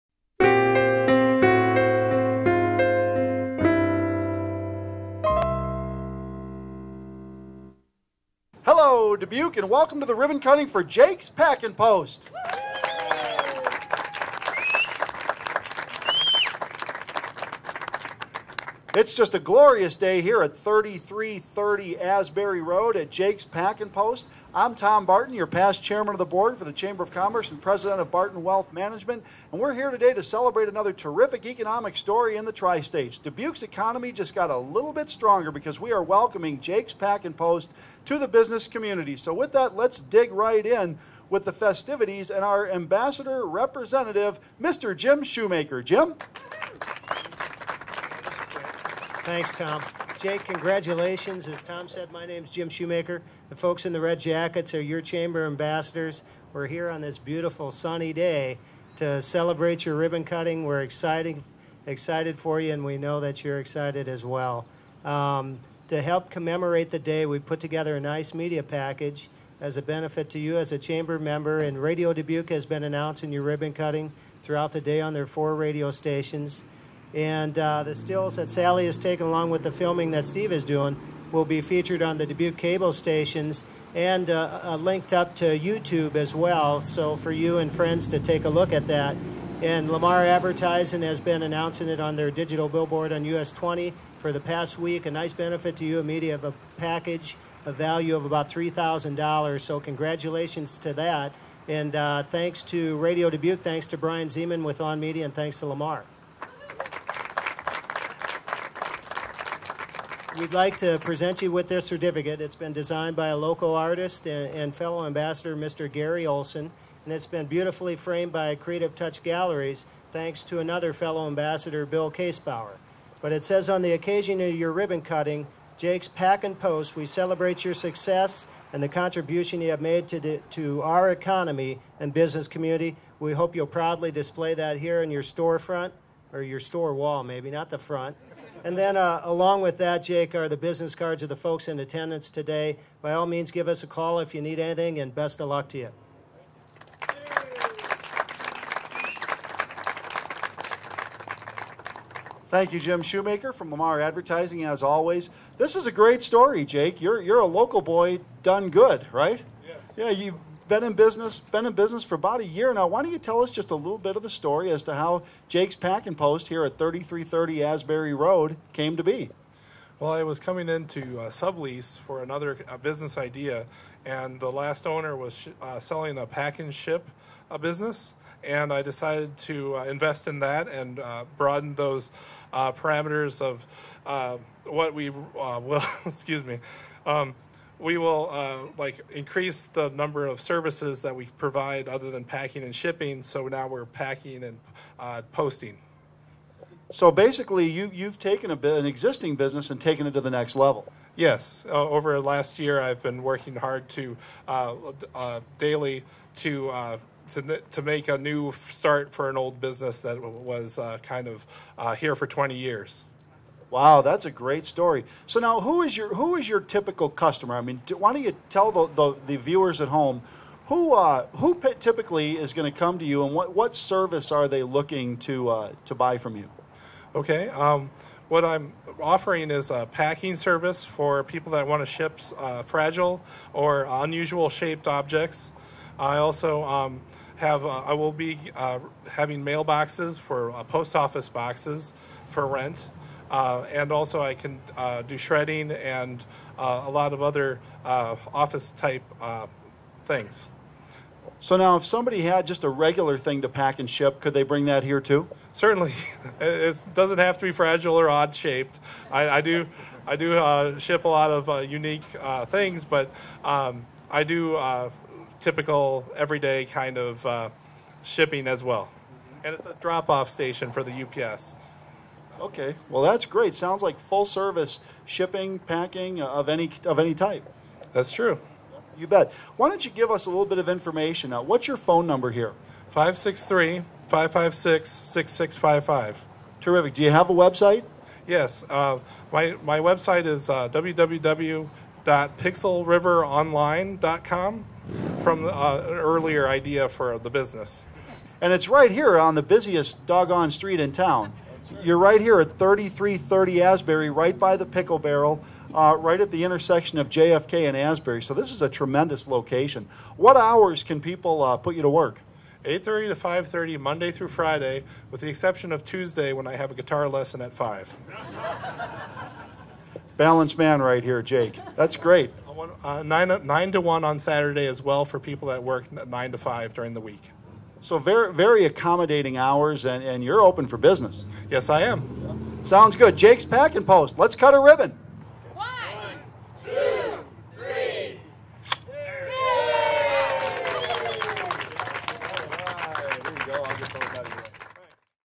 Jake's Pack N Post
Jake's Pack N Post - 3330 Asbury Rd. Dbq. Ia.563-556-5655 owner Jake Robertson celebrates riboon cutting with Chamber ambassadors. Jake's Pack N Post will pack and send any size or shape item and send it via UPS to its destination. find Jake